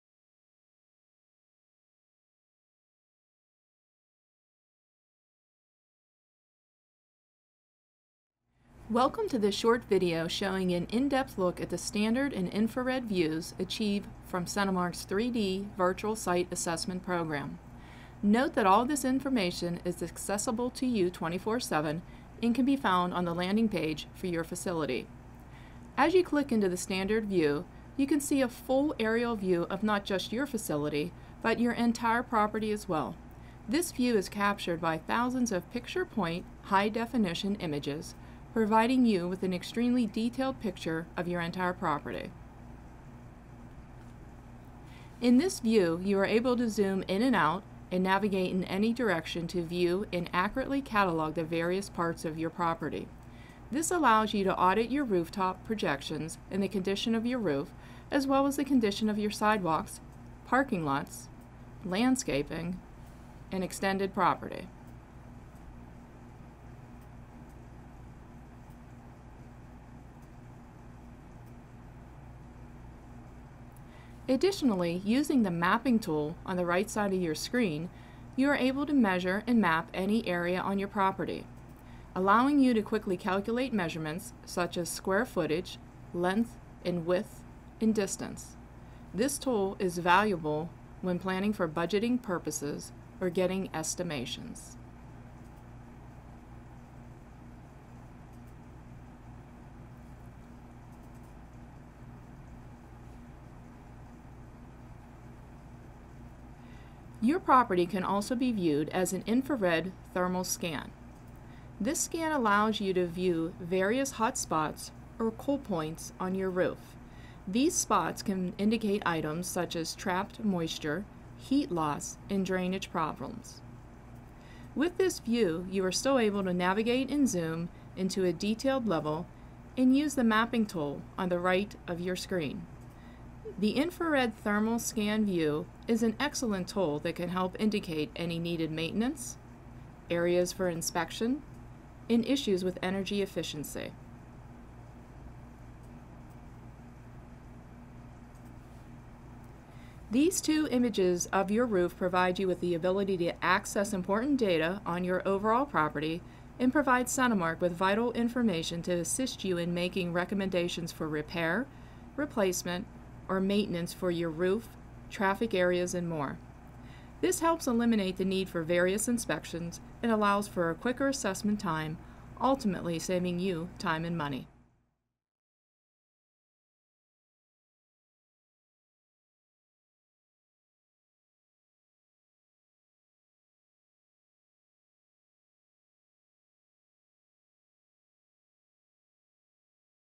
Why use thermal imaging on flat roofs?
Check out this video to see how CentiMark can inspect your roof with innovative technology! Our 3D Virtual Site Assessment Program utilizes state-of-the-art technology and drones with high definition, infrared and thermal imaging capabilities to safely obtain an entirely new perspective when inspecting your entire property. The standard view and thermal scans provide the ability to navigate in any direction to get a 3D view of your building and see variances in temperature, moisture, leaks and more on your roof.

To learn more about our 3D Virtual Site Assessment Program or to schedule an assessment for your property,